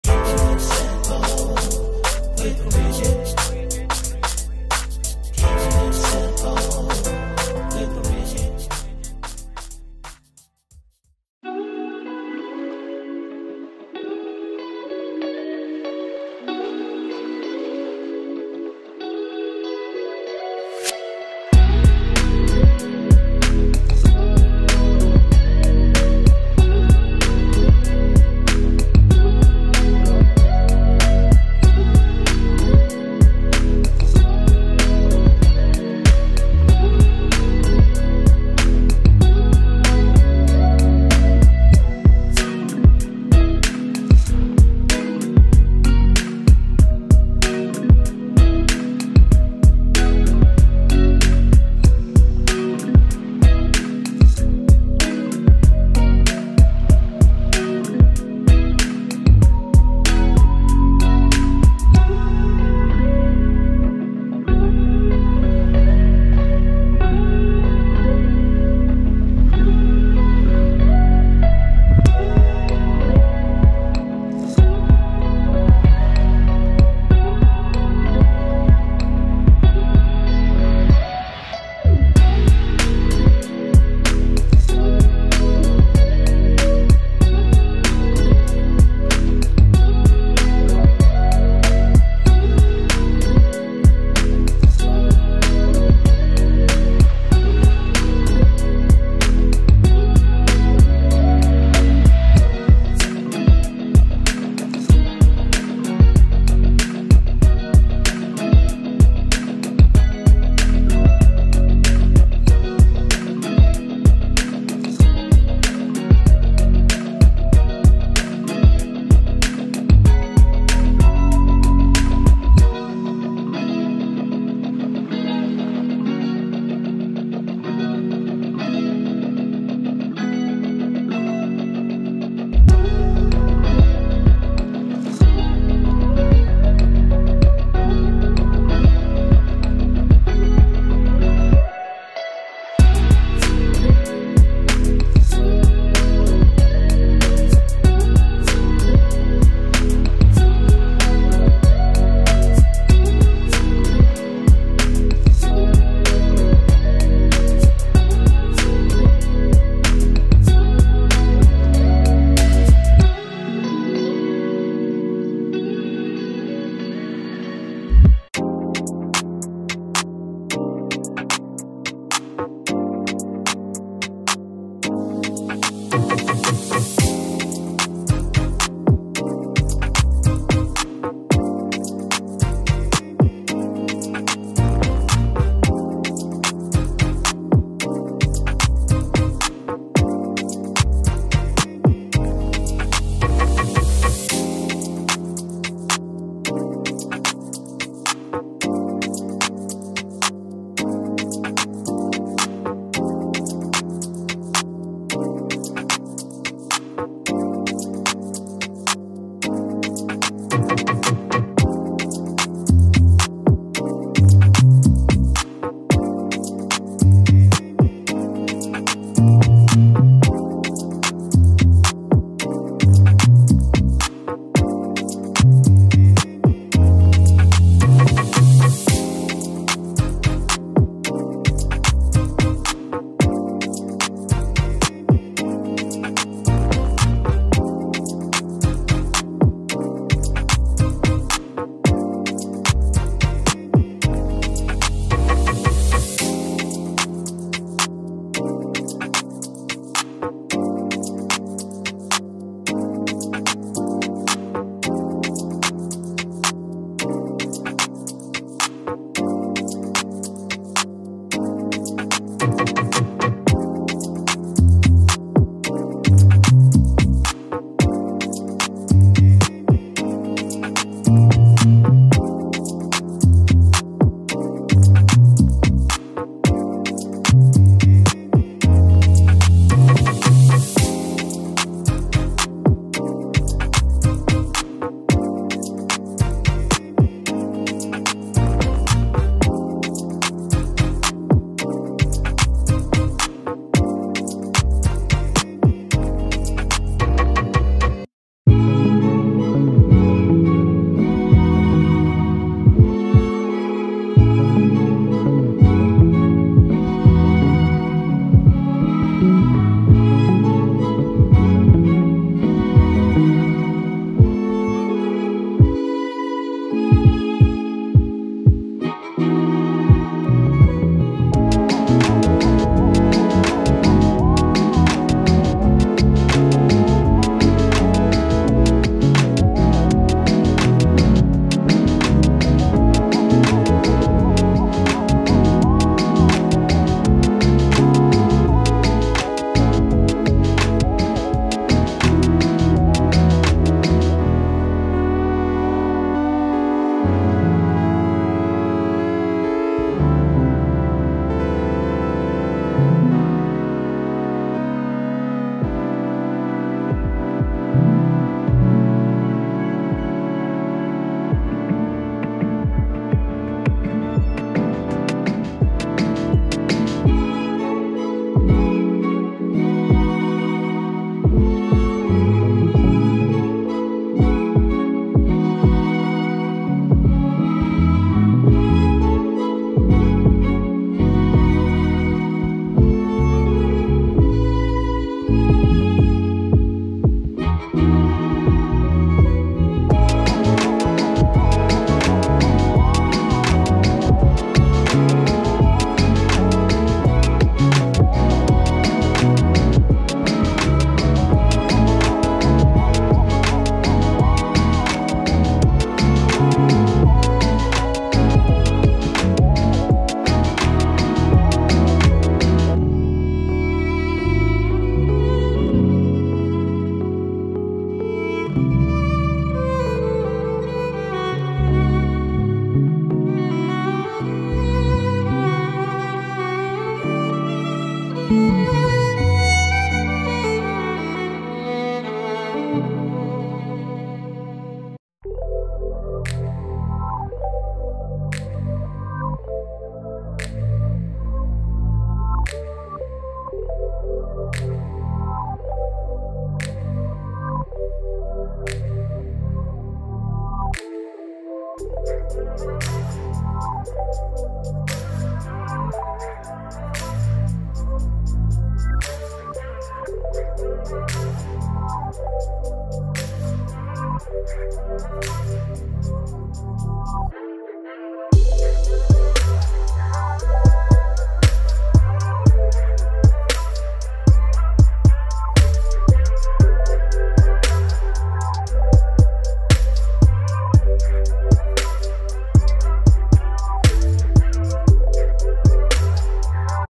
New Morphe Desert Bouquet Artistry Palette ☆ 4 Looks One Palette
Hello everyone! I am back with another video. This time I have the new Morphe 35D Desert Bouquet Artistry palette with 4 Looks. Please let me know which one you like. Also, did you pick this one up or did you pass on it?

Time Stamps
Look #1 -  2:18
Look #2 - 3:38
Look #3 - 5:57
Look #4 - 7:57

Keeping it Simple with Bridget